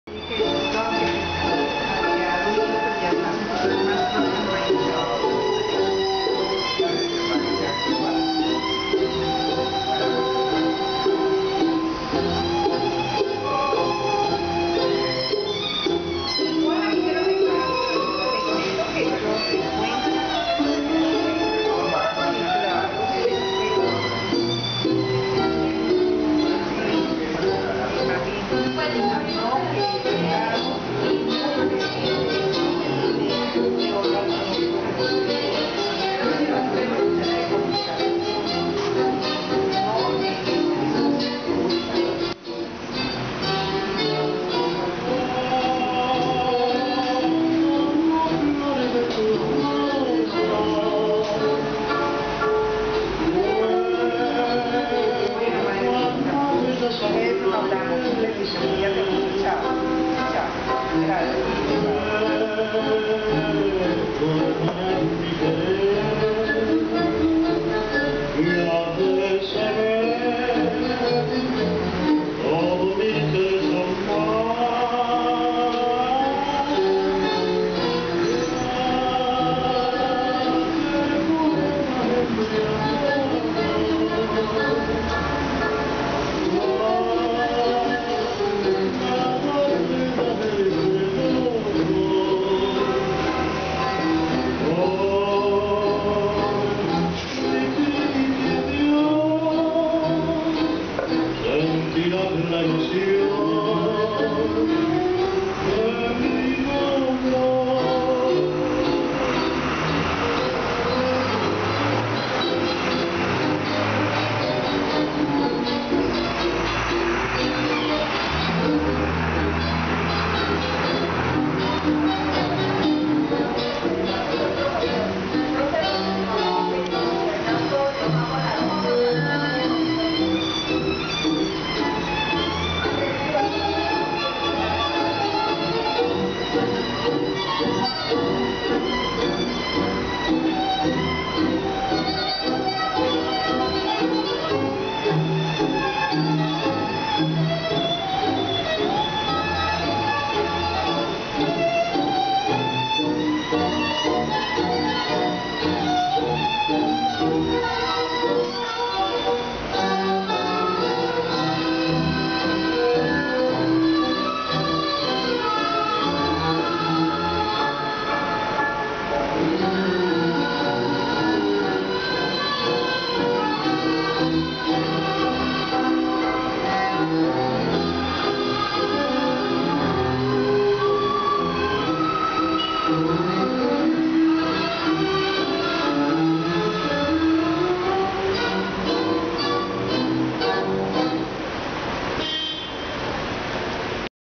Dancing tango in Cali, second trip
I'm so lucky that I started my tango journey with a world champion in scenario in 2010 in Cali, Colombia, the world capital of salsa. I went back in September 2012 and took classes with Carlos Paredes again.